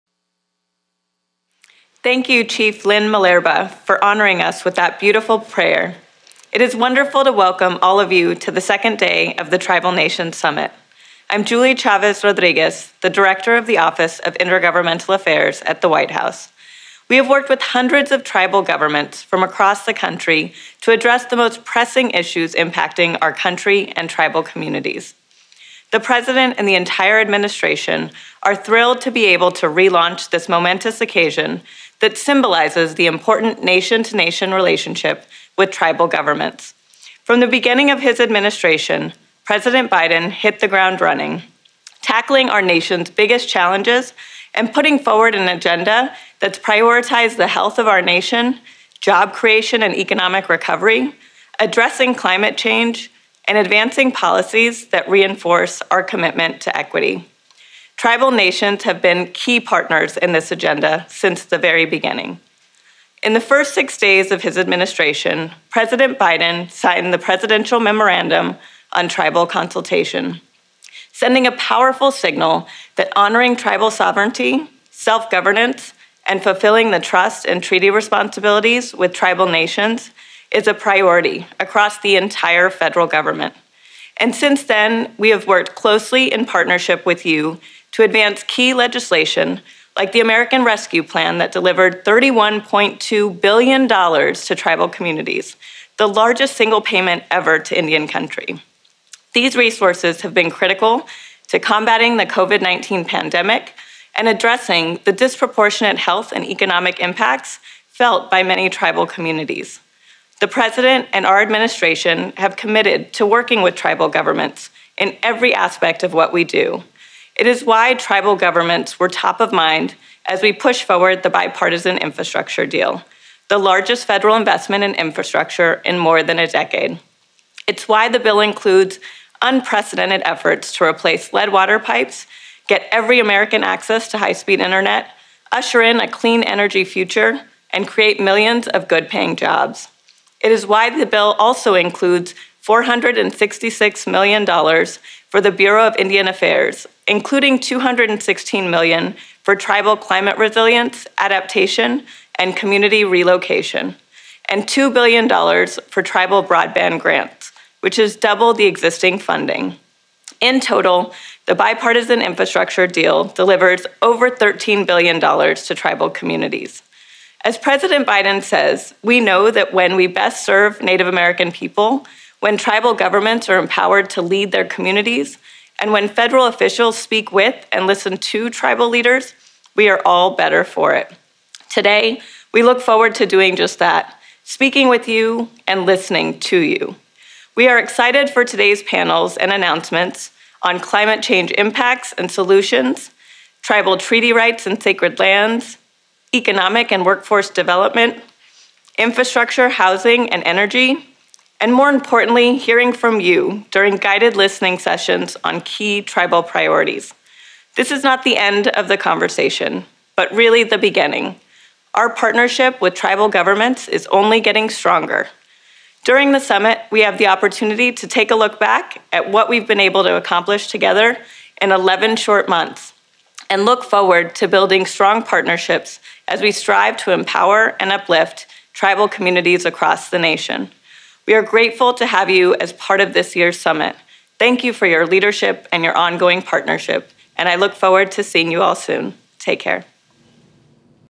Julie Rodriguez - White House Tribal Nations Summit - November 16, 2021
Julie Rodriguez addresses the White House Tribal Nations Summit on November 16, 2021.

Rodriguez serves as director of the Office of Intergovernmental Affairs at the White House. The office helps tribal, local and state governments work with U.S. agencies.

Video courtesy White House